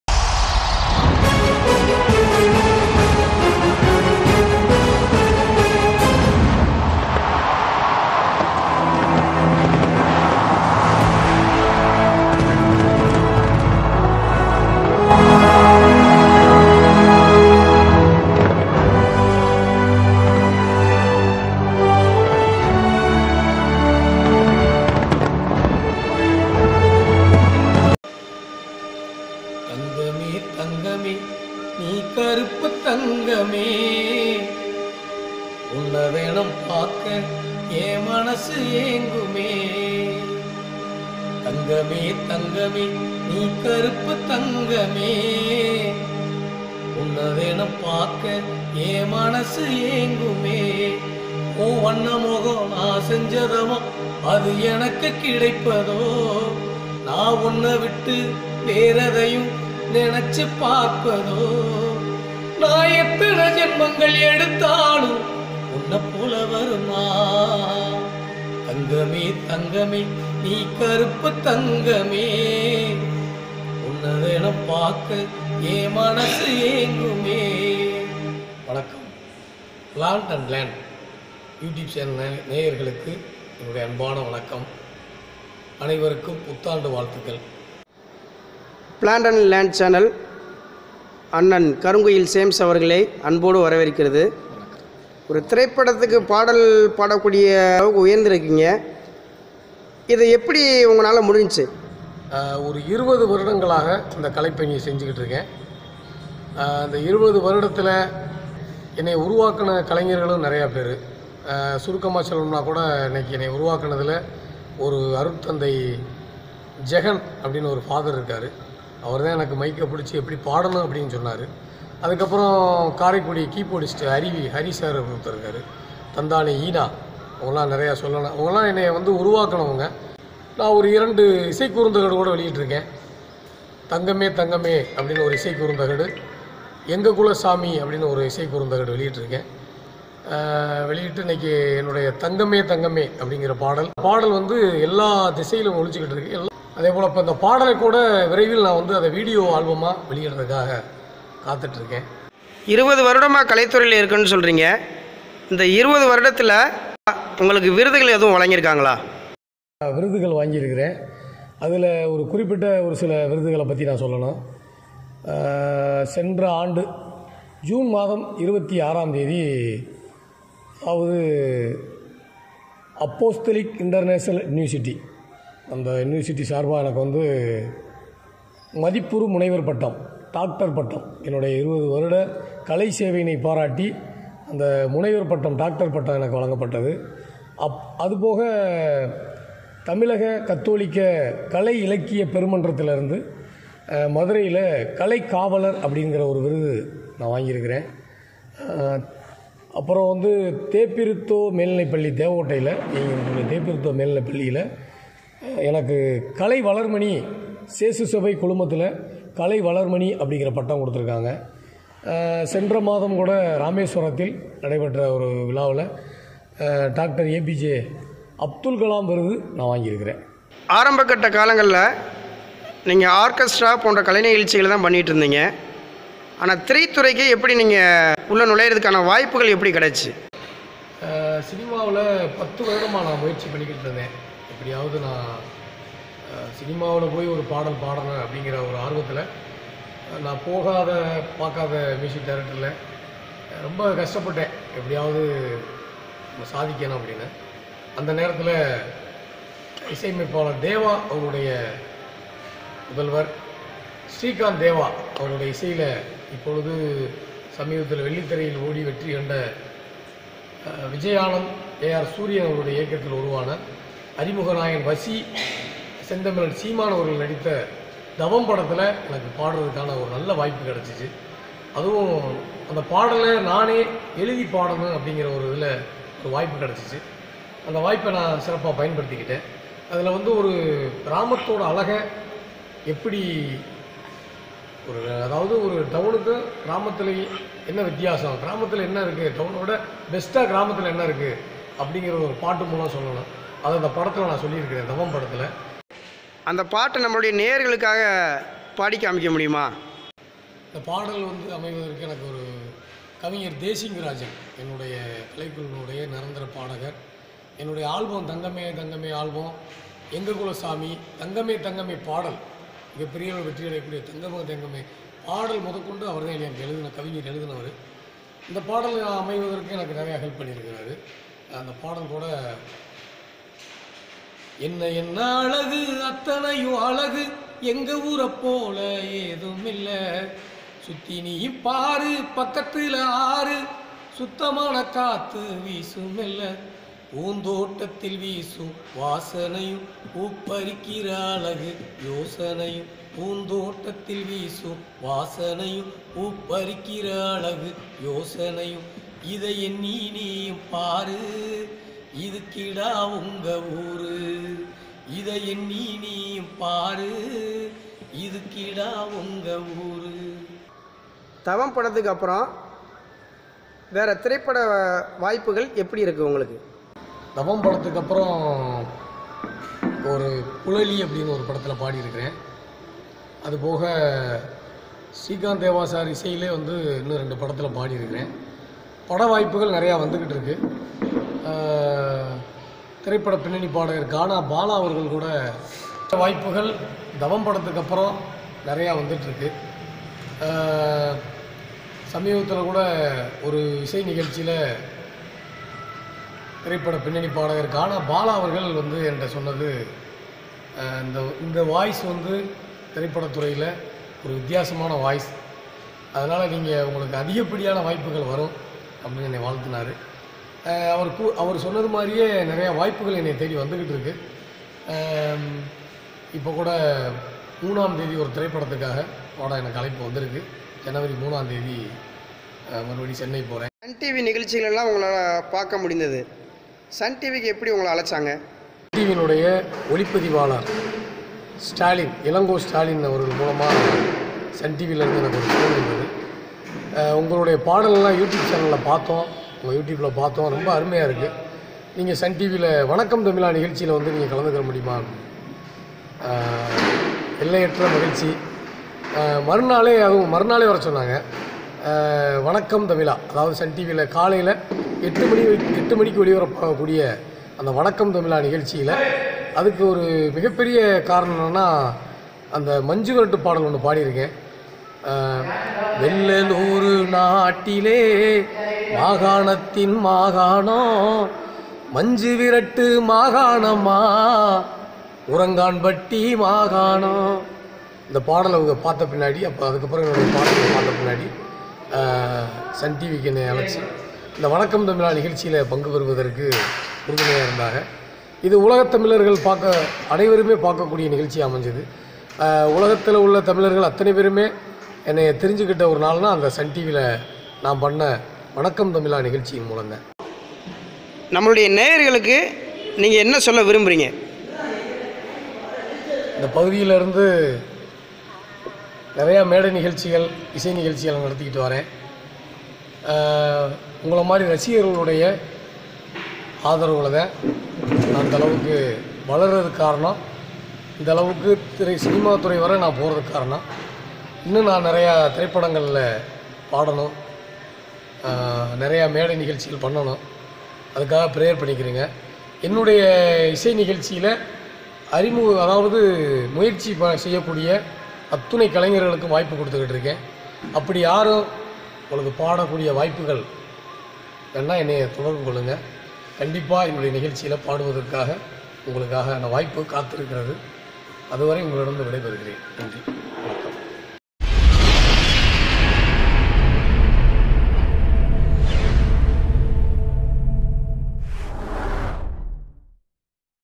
திரைபட பாடகர் ஜேம்ஸ் - 2020 Interview நேர்காணல்
Plantandland திரைப்பட பின்னணி பாடகர் கருங்குயில் ஜேம்ஸ் தனது முயற்சியால் 20 ஆண்டுகாலம் கச்சேரிகளை நடத்தி இன்று மிகப்பெரிய ஒரு இலக்கை அடைந்துள்ளார்.சிறிய குக்கிராமத்தில் பிறந்து பல கஷ்டங்களை கடந்து திரைப்படத்தில் பாடல்களைப் பாடியுள்ளார்.இன்று அவருடைய கடின முயற்சியால் பல விருதுகளையும் பட்டங்களையும் பெற்றுள்ளார்.நாட்டுப்புற கலைகள் அழியாமல் இருக்க இதுபோன்ற கலைஞர்களே காரணமாக உள்ளனர்.filmmaster#